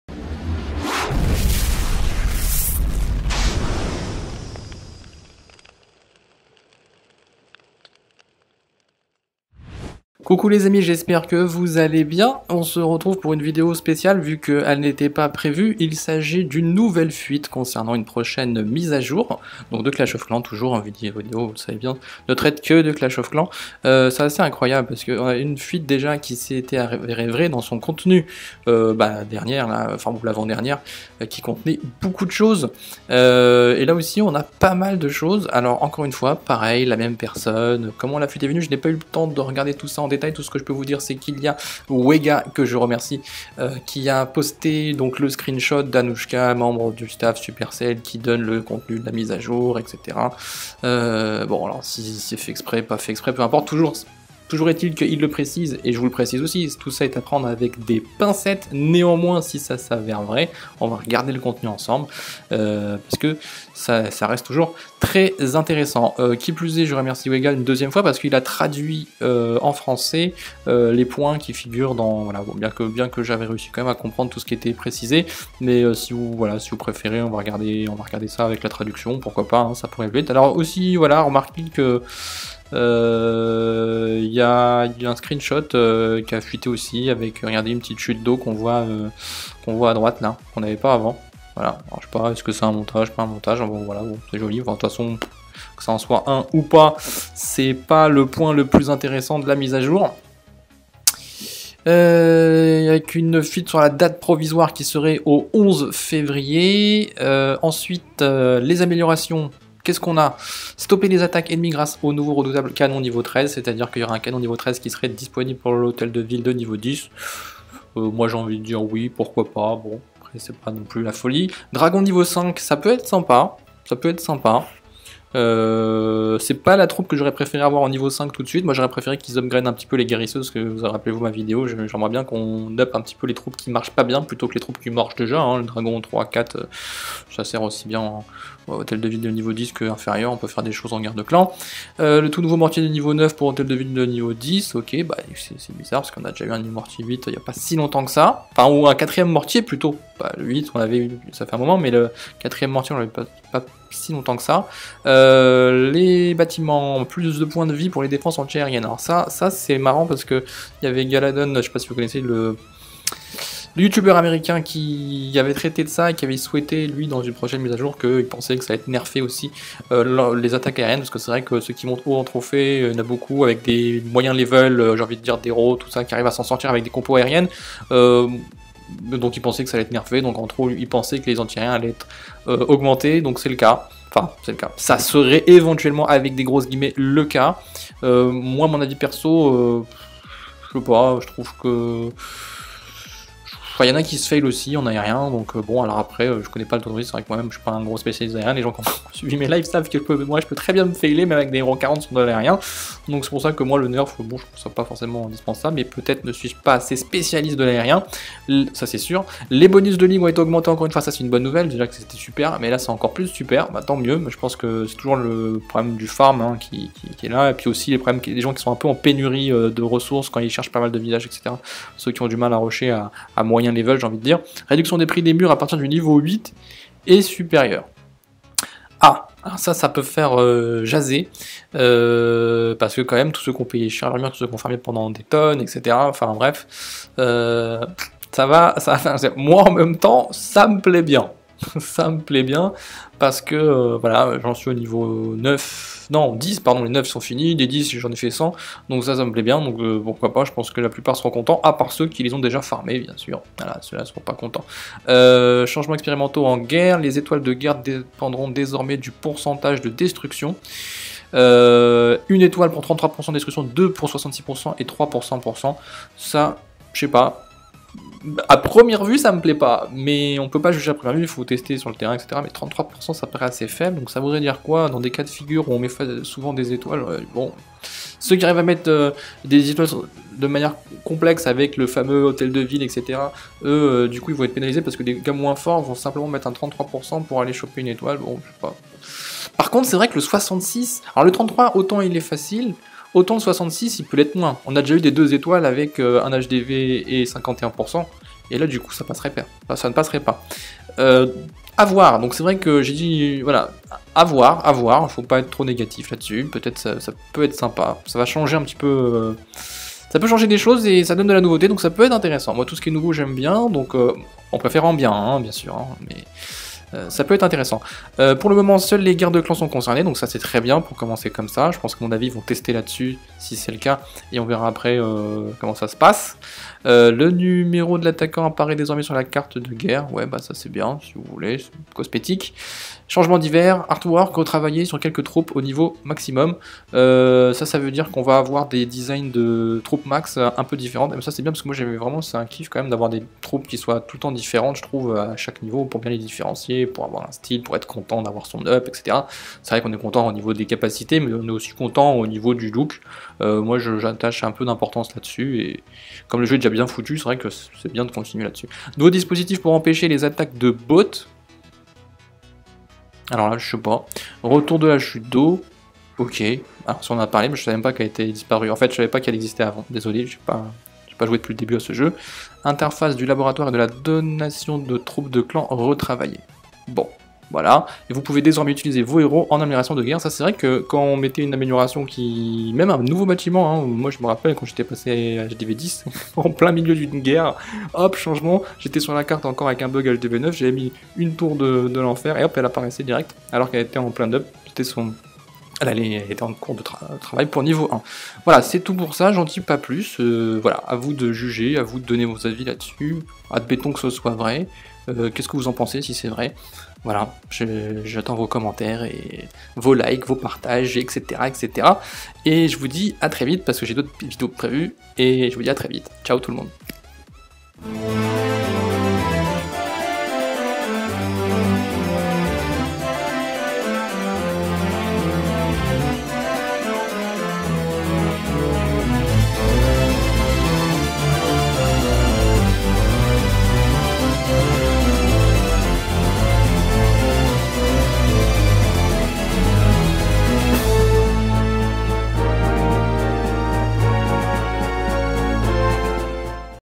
Fuite ! Le contenu de la future mise à jour dévoilée ?! | Clash Of Clans Français
Le contenu de la future mise à jour serait-il déjà dévoilé ???
Vos commentaires en actions !

★ ★ Suis mes aventures !